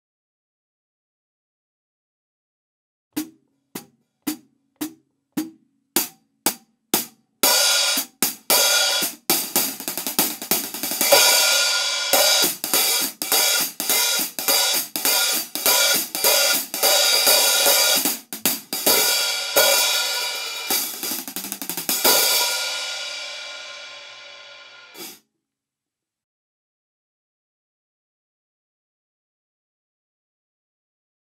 14” swing king hi-hat - SKH14 NEW
No EQ or compression was used at recording or mastering stages. All recording and mastering were done in the digital domain. The recordings will give you a basic idea of TC models and series. We recommend using headphones for a better sound quality.

Gear List:
Room mic: Shure KSM44
Preamp: Avalon VT-737sp
Sticks: Vater 8A wood tip & 5B wood tip
Accessories: Cympad washers

Performed by Arbak Refik Dal